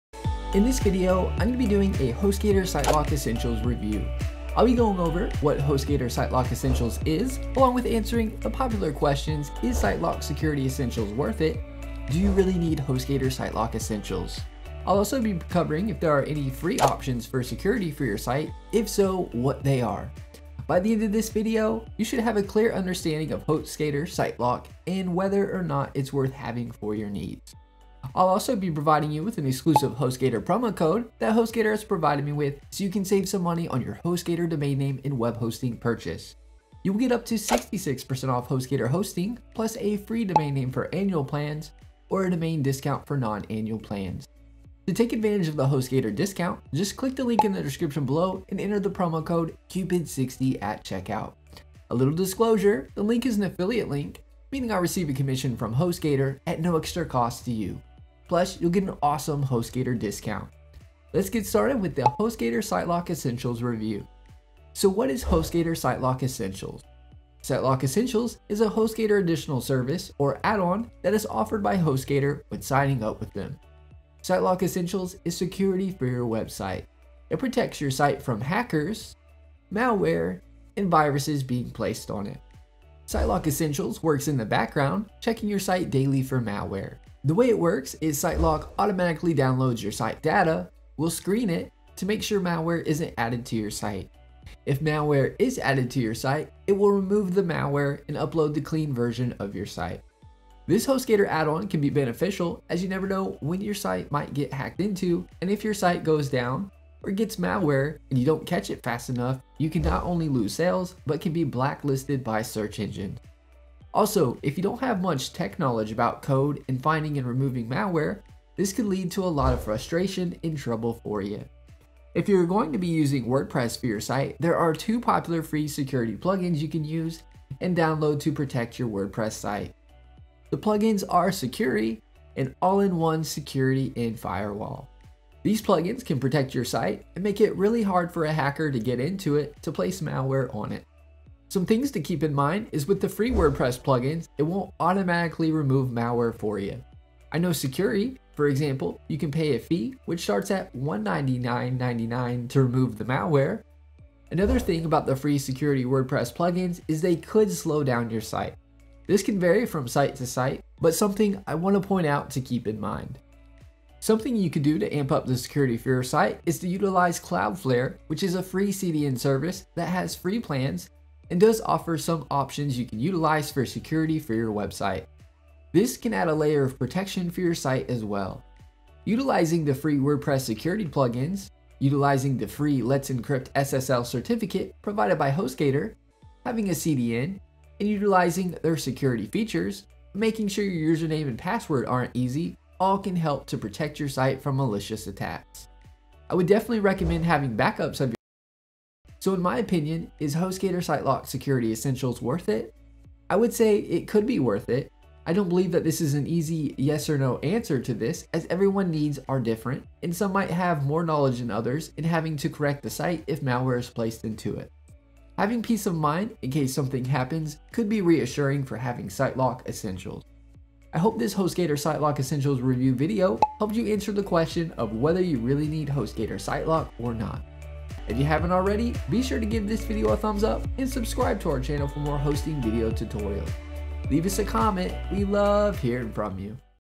Is Hostgator Sitelock Essentials Worth It? Do You Need It? (Review)
In this Hostgator Web Hosting tutorial, we go over the Hostgator additional service Hostgator Sitelock Essentials.

In this Hostgator Sitelock Essentials Review video I'll go over:
What Is Hostgator Sitelock Essentials?
Is Hostgator Sitelock Essentials Worth It?
Do You Need Hostgator Sitelock Essentials?

Along with the three questions I'll be giving my honest Hostgator Sitelock Essentials Review and Opinion to see if it is worth having for your site needs.

The Hostgator Sitelock Essentials add-on can be beneficial as it will protect your site from hackers and malicious attacks. It can also remove malware. There are 2 free WordPress security plugins called Sucuri and All In One Security And Firewall. These do a good job with security for your site. The only downfall is malware removal isn't included. You can pay to have it removed. For example, Sucuri has a paid yearly package you can pay that will cover this.

Is Hostgator Sitelock Essentials Worth It? In my honest opinion it is worth having. Especially if you don't have any tech knowledge and dealing with code. If you are familiar with code or tech stuff you might find it not being worth it. I don't believe there is a straight yes or no answer as everyones knowledge and needs are different.